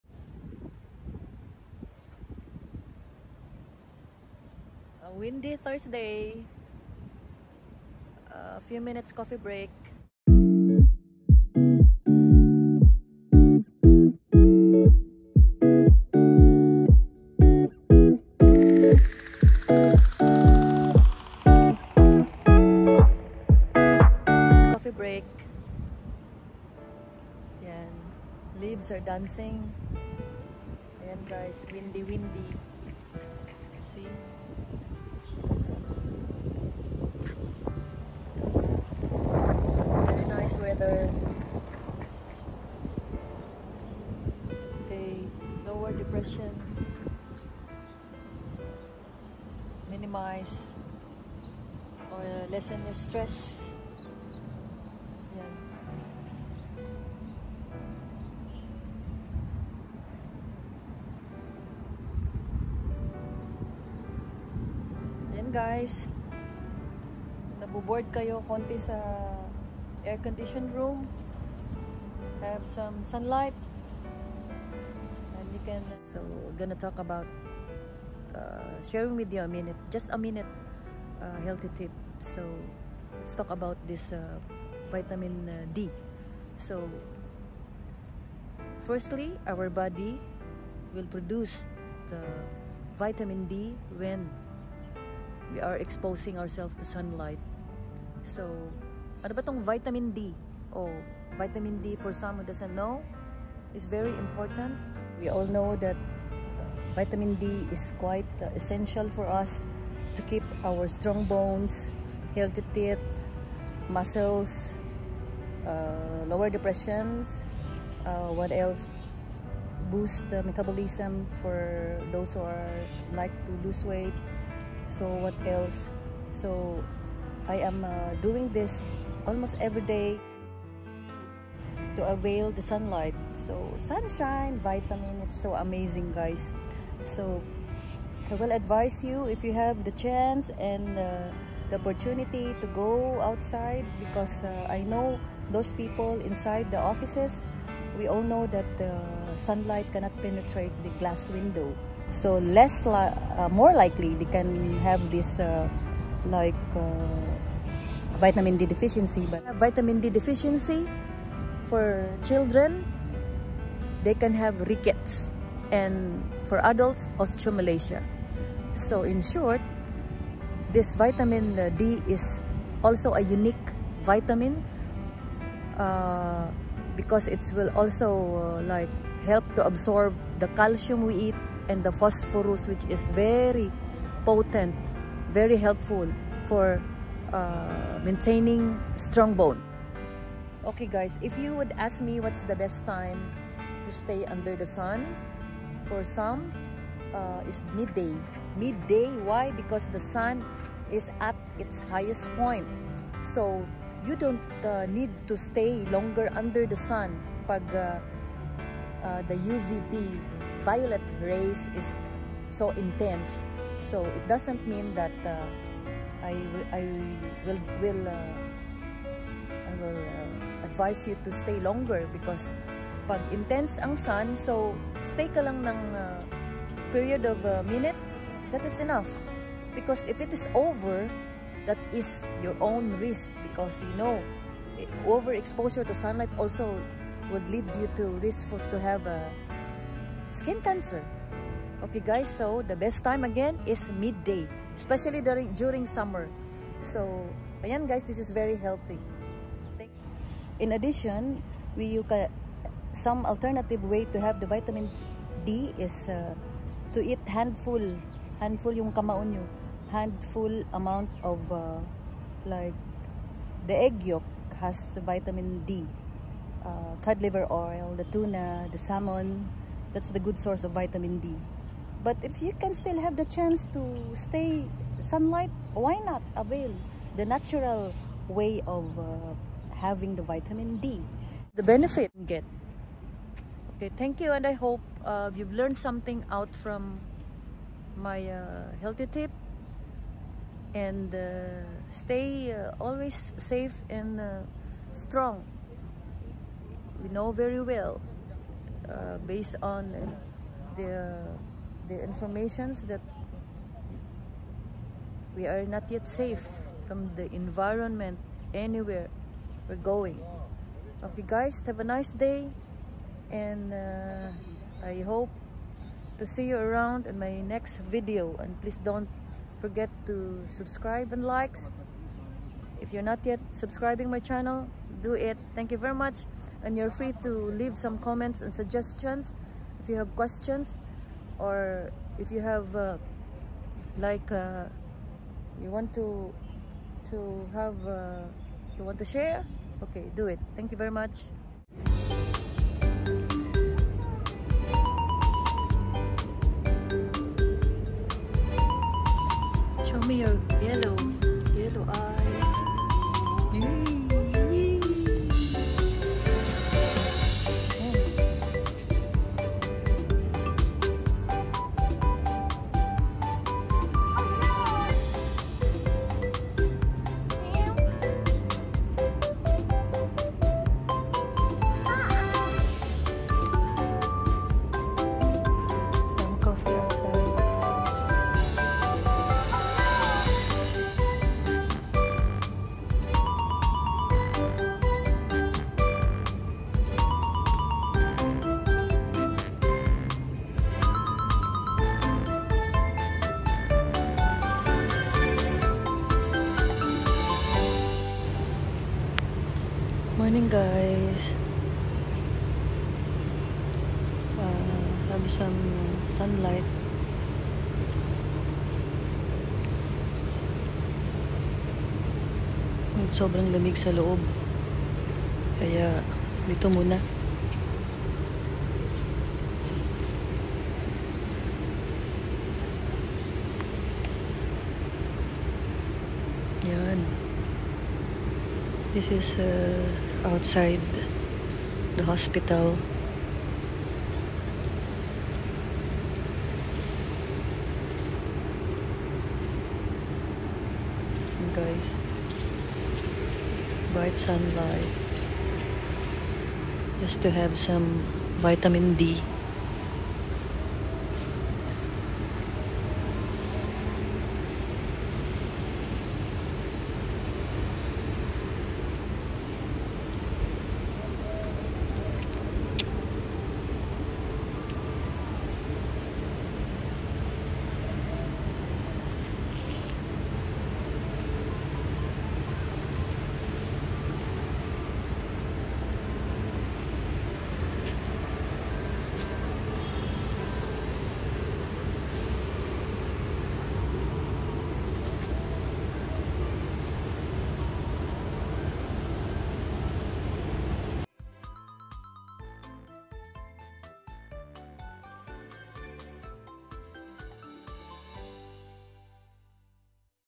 SUNLIGHT Breaktime Vitamin D
When your skin is exposed to sunlight, it makes vitamin D from cholesterol. The sun's ultraviolet B (UVB) rays hit cholesterol in the skin cells, providing the energy for vitamin D synthesis to occur.
Good sources of vitamin D
oily fish – such as salmon, sardines, herring and mackerel.
red meat.
liver.
egg yolks.
fortified foods – such as some fat spreads and breakfast cereals.